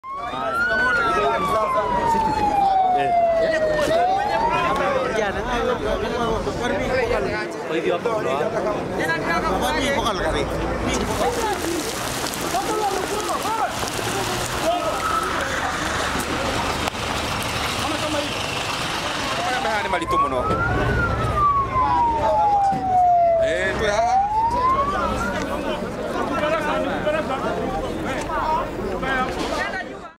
Deadly road accident kills dozens in Kenya
At least 48 people were killed in a road accident in Londiani, western Kenya, when a lorry carrying a shipping container veered off the road and ploughed into several vehicles.